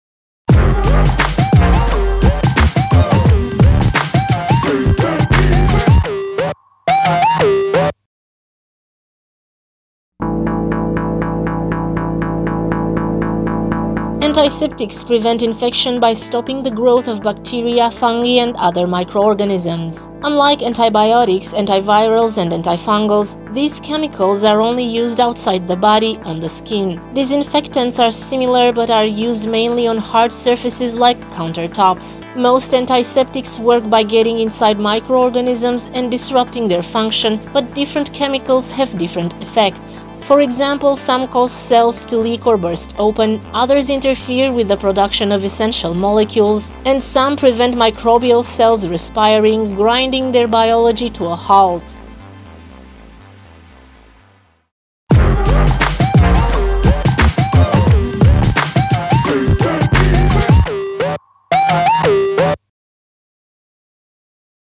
How do antiseptics work?
Unlike antibiotics, antivirals and antifungals, these chemicals are only used outside the body, on the skin.